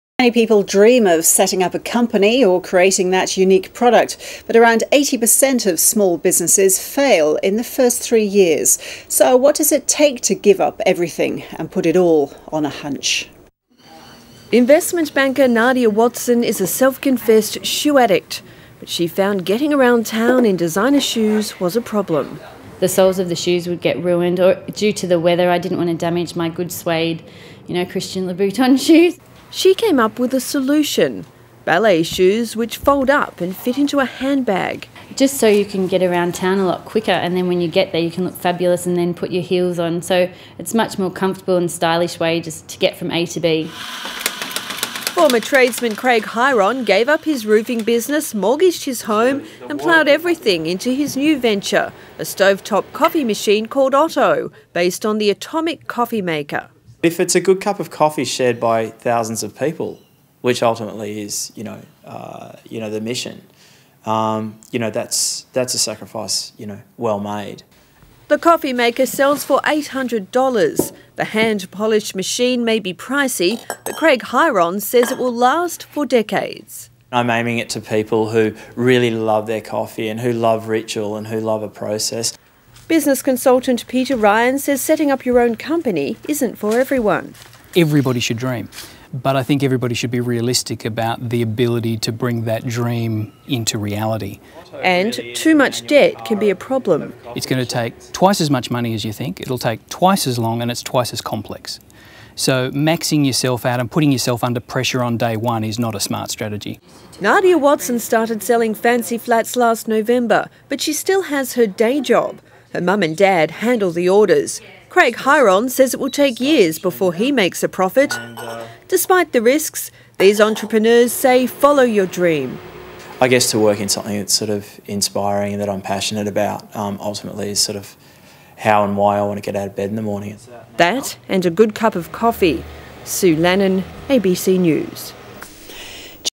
Otto Espresso - ABC TV interview
An interview with Craig Hiron on starting up Otto Espresso from Australia's ABC TV business show.